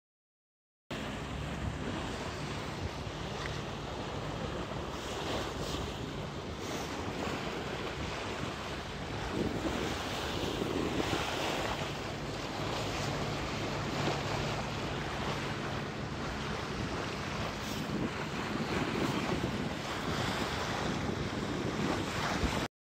Motoring to Lymington
May Bank Holiday and no wind. So motored to Lymington from Southampton, on a scorchio day With Barry and Mick